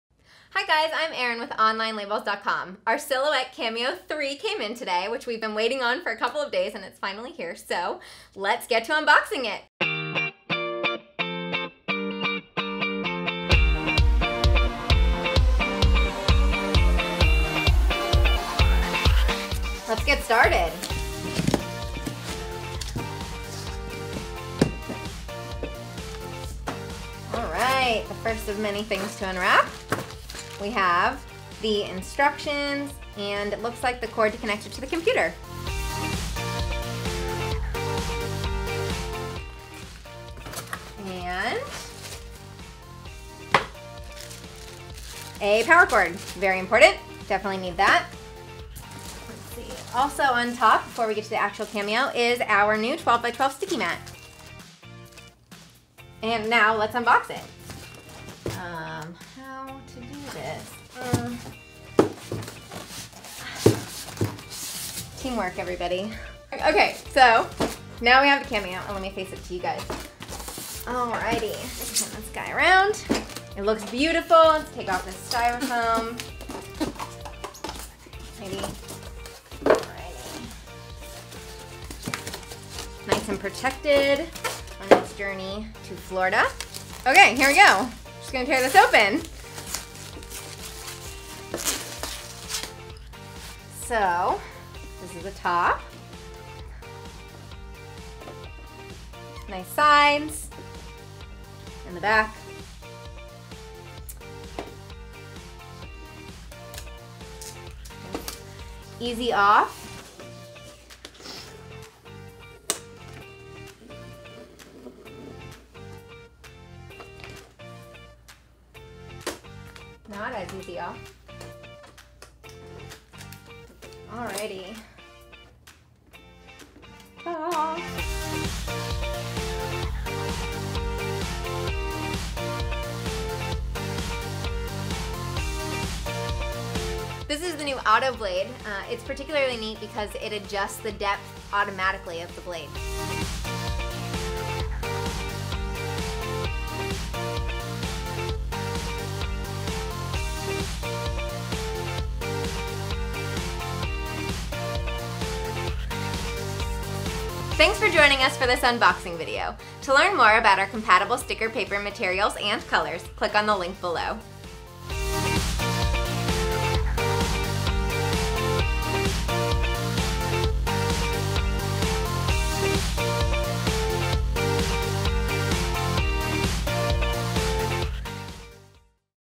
Silhouette Cameo 3 Unboxing - See What's Inside
Looking to buy a Silhouette Cameo 3? Watch this unboxing so you can see what to expect when you buy one. This machine is great for cutting a wide variety of sticker paper materials. And guess what? We sell a wide variety of sticker paper materials! Get started shopping below: